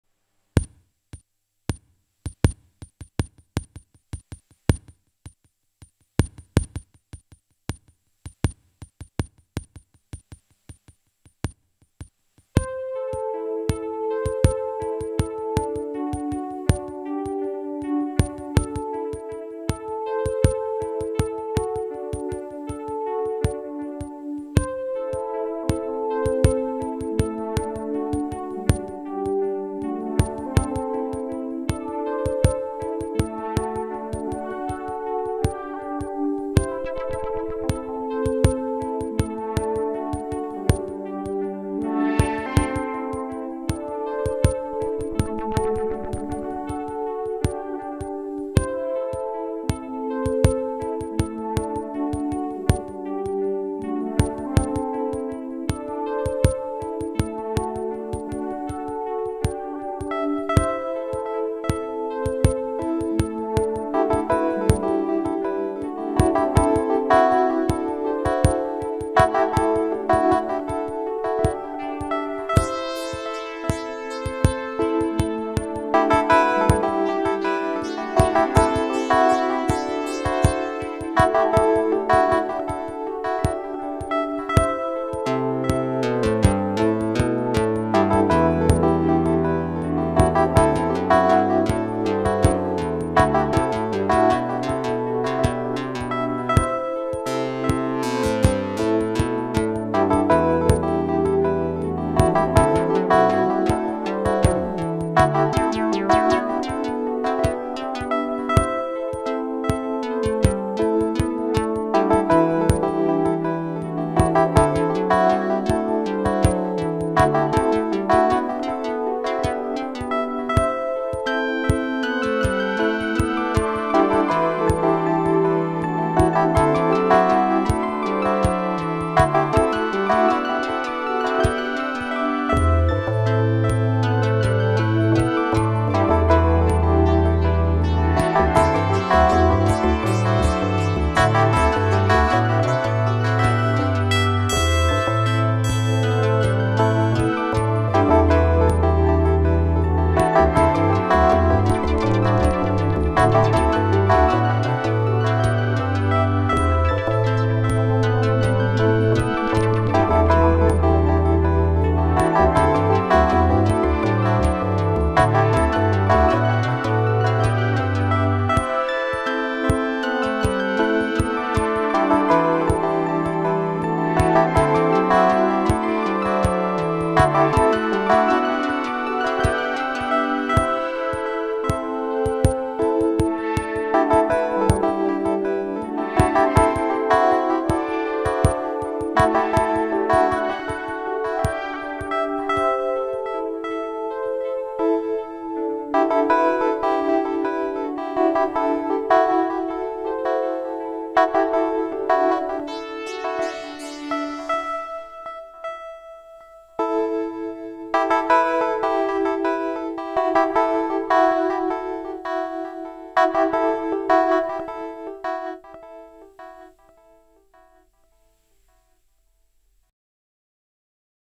Jamuary 15, 2023. Jam. Smooth Jam.
My hands (initially). Elektron Digitakt, Digitone, and Analog Heat. Oberheim OB-X8. Moog Matriarch. Novation Peak. Dreadbox Typhon.